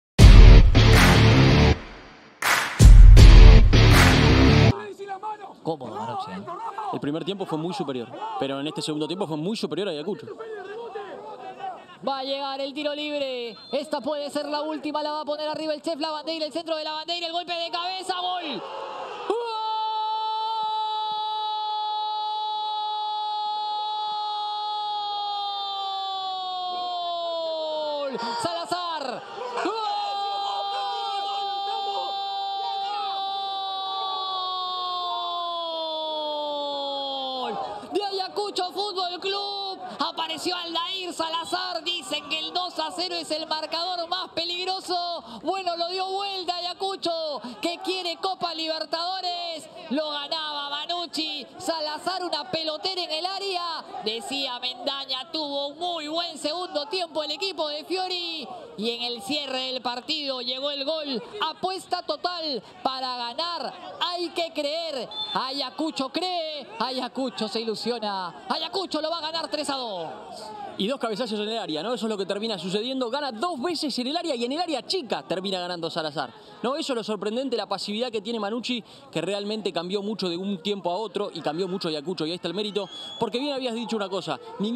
Liga1 Betsson: Aldair Salazar anotó así el Ayacucho FC 3-2 Carlos A. Mannucci
¡LO DIO VUELTA! Aldair Salazar 🔛🇵🇪 anota en el último minuto ⏱ y coloca el tercero para Ayacucho FC ante Carlos A. Mannucci para llevarse el triunfo 3-2 🔥 por la fecha 14 de la Fase 2 de la LIGA1BETSSONXGOLPERU 🏆🇵🇪.